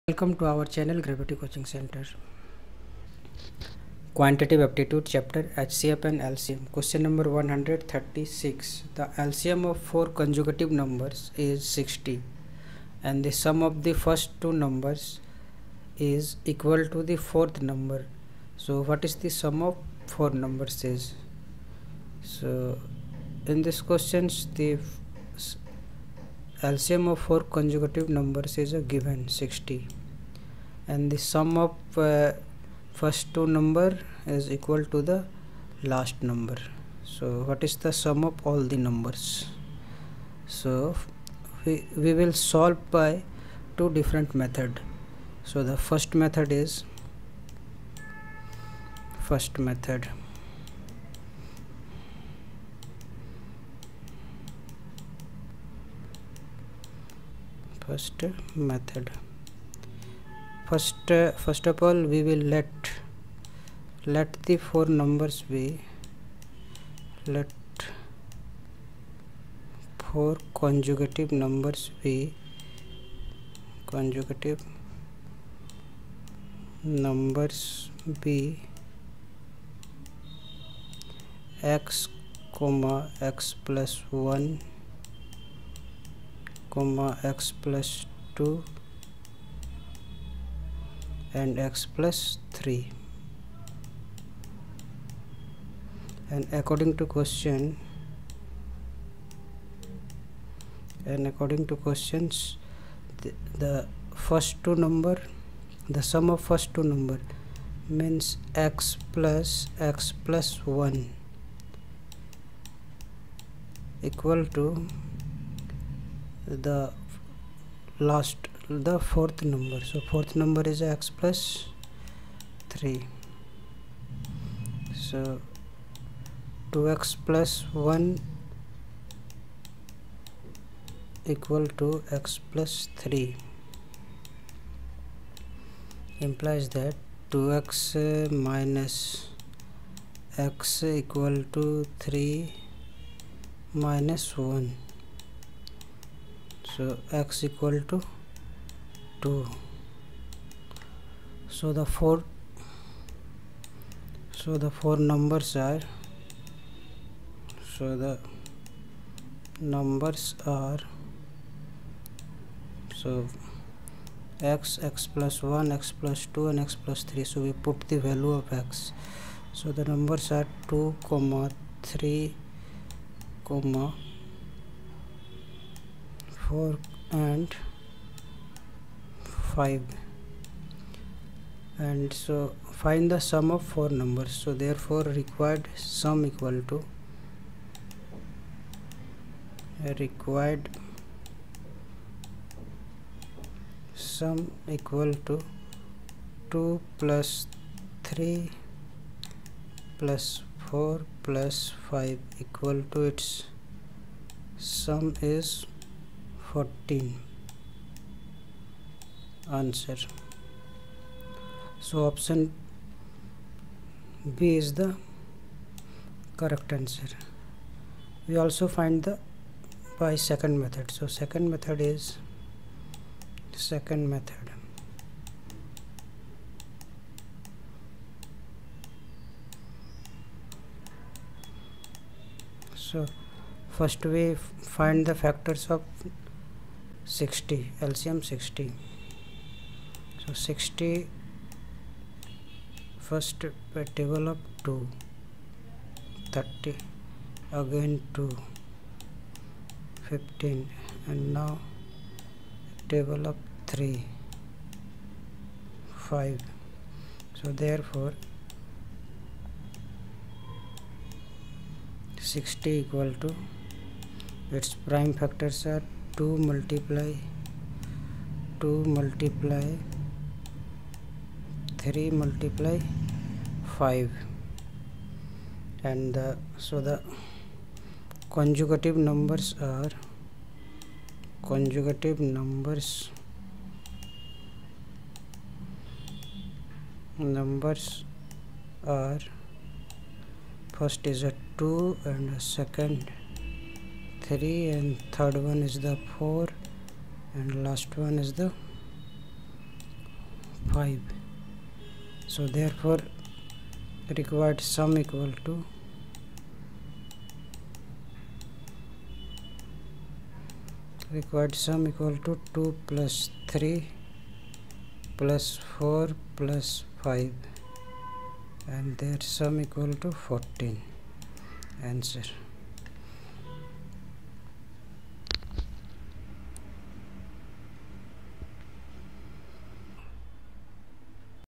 Q136 | The LCM of four consecutive numbers is 60. The sum of the first two numbers is equal to the
Click on below Links for more Questions:-
Qu(136):- The LCM of four consecutive numbers is 60. The sum of the first two numbers is qual to the fourth number. What is the sum of four numbers?